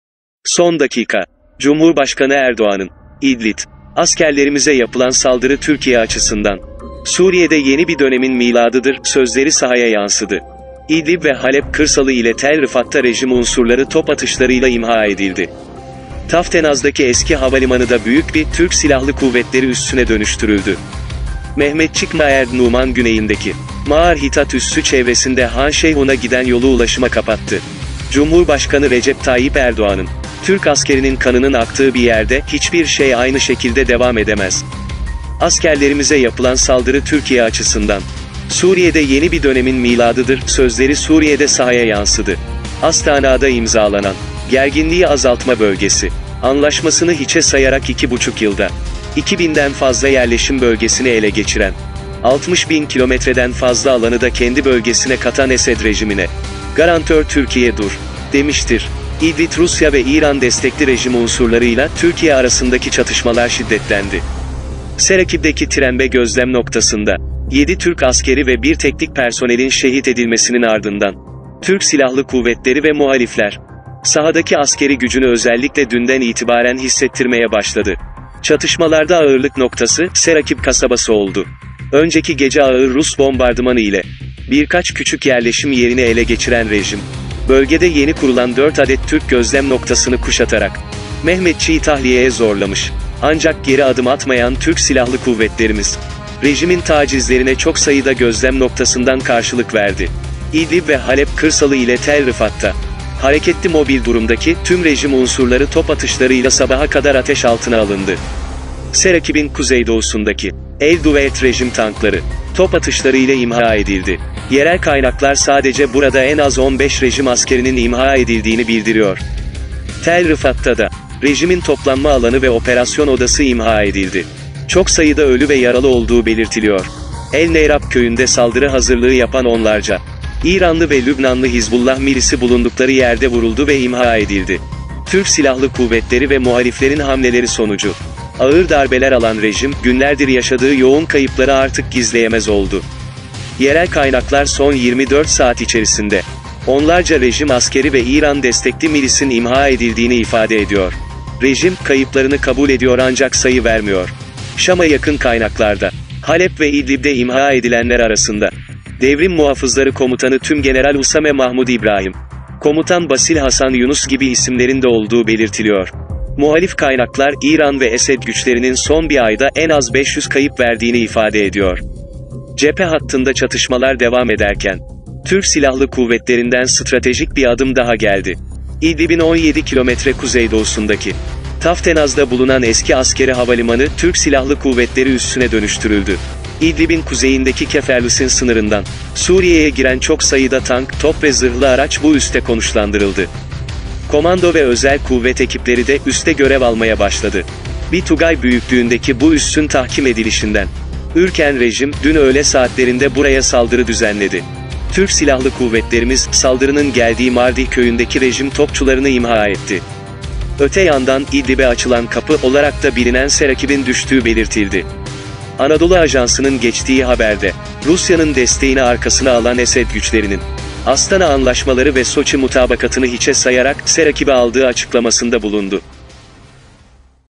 Türk Silahlı Kuvvetleri’nden Esed Rejim Askerine Darbe Çok Sayıda Tank İmha Edildi
Rus bombardımanı ile birkaç küçük yerleşim yerini ele geçiren rejim, bölgede yeni kurulan 4 adet Türk gözlem noktasını kuşatarak Mehmetçiği tahliyeye zorladı. Ancak geri adım atmayan TSK, rejimin tacizlerine çok sayıda gözlem noktasından karşılık verdi. İdlib ve Halep kırsalı ile Tel Rıfat’ta hareketli/mobil durumdaki tüm rejim unsurları top atışlarıyla sabaha kadar ateş altına alındı. Serakib’in kuzeydoğusundaki El Duveyr’de rejim tankları top atışlarıyla imha edildi; yerel kaynaklar sadece burada en az 15 rejim askerinin öldüğünü söylüyor.